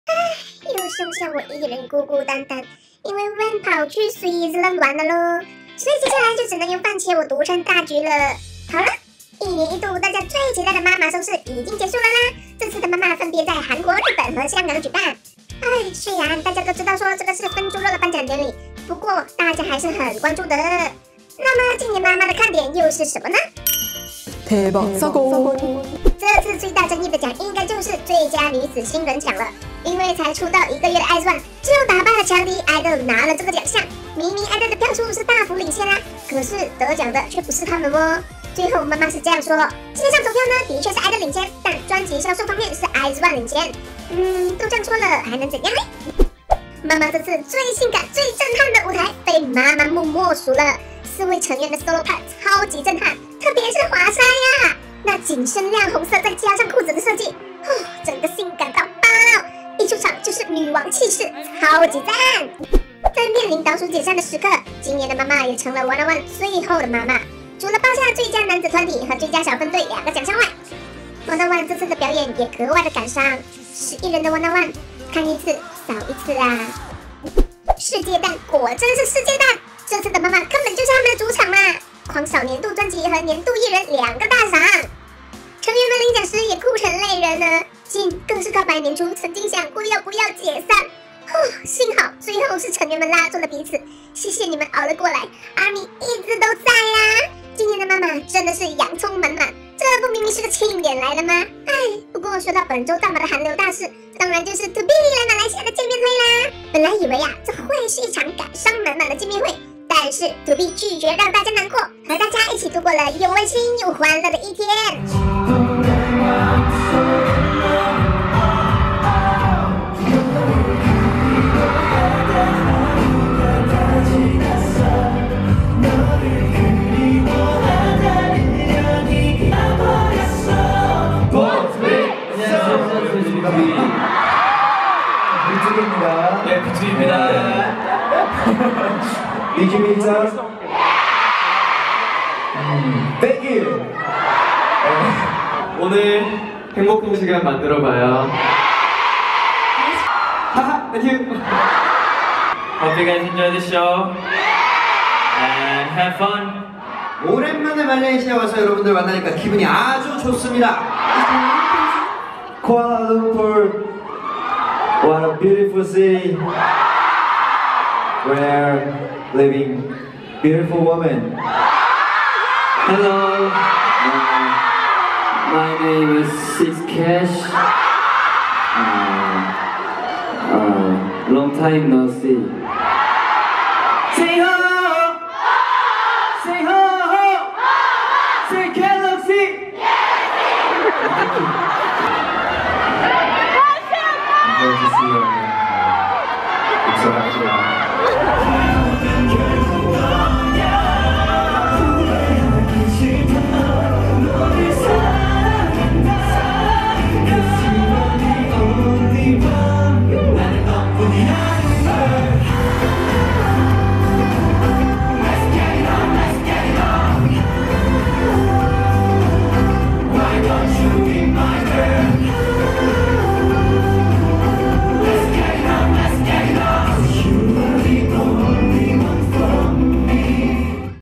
韩流Channel EP77：MAMA看点数一数 + BTOB见面会超欢乐啊！
🍅FUN茄：MAMA盛事结束啦！不过我们还有兔逼啊！
来看看见面会上幽默风趣的他们吧！